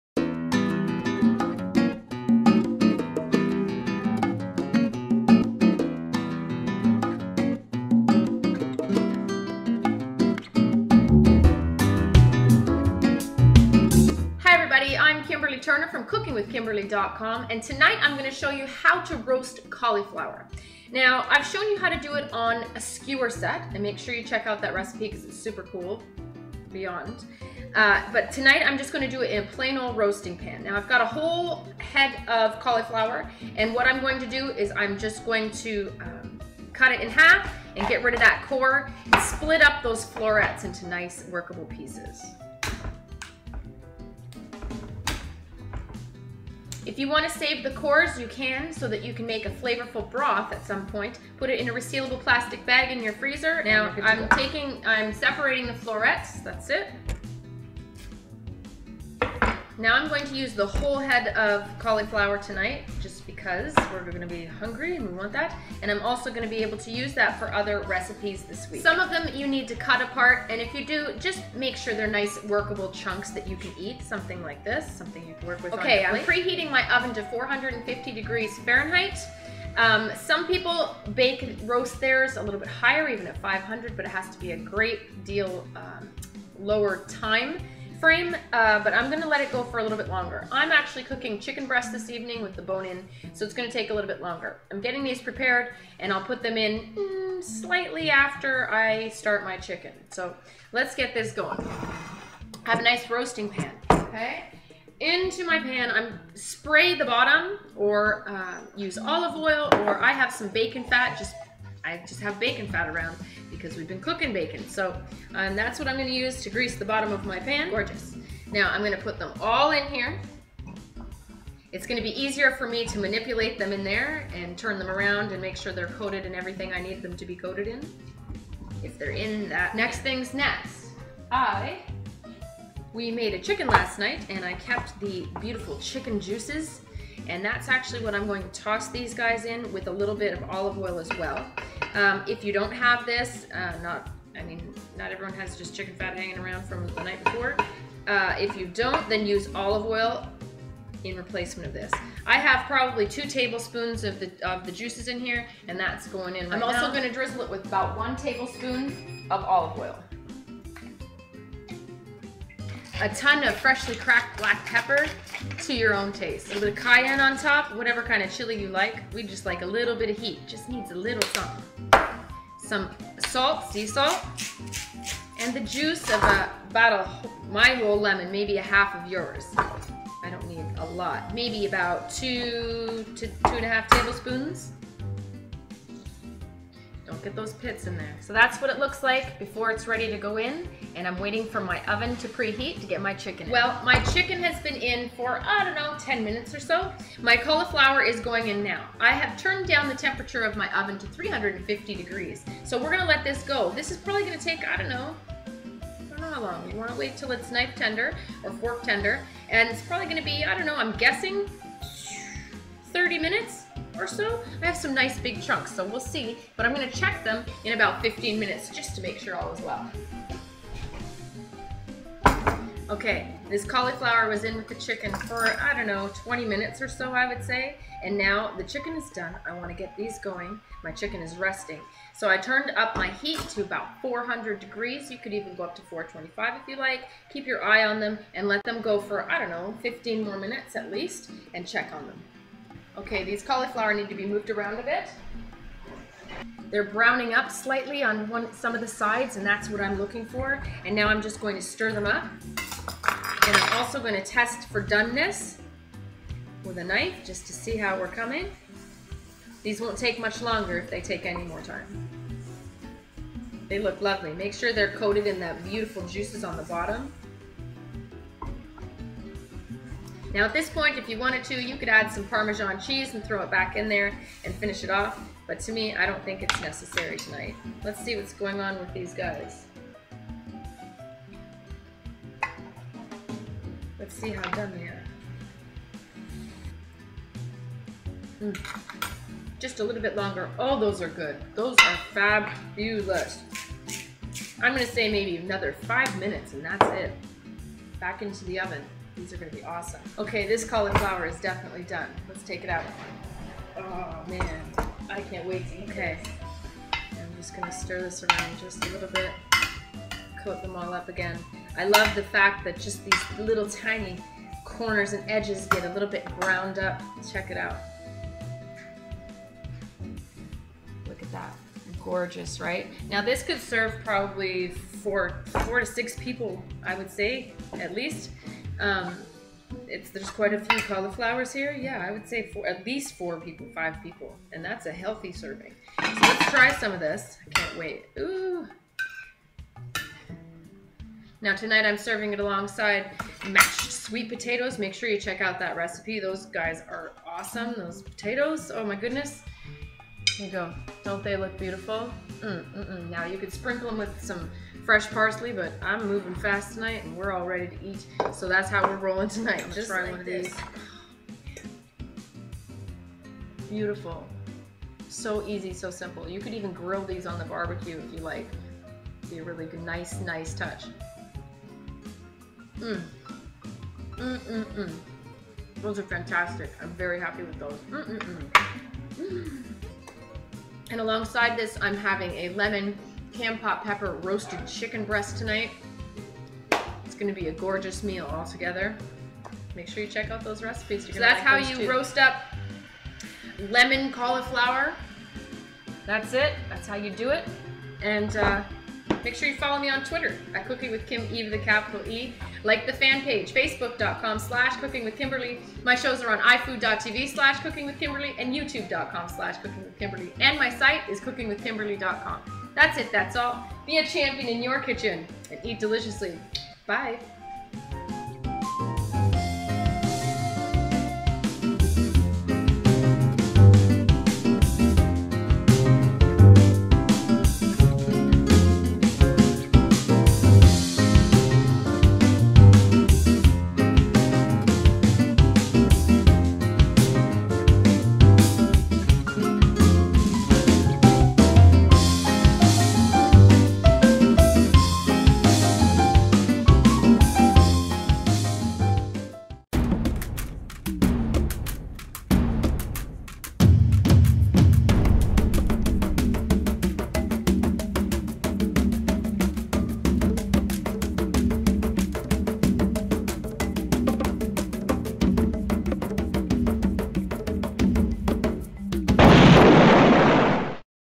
How to Roast Cauliflower with Lemon: Cooking with Kimberly
shares with you How to Roast Cauliflower with Lemon!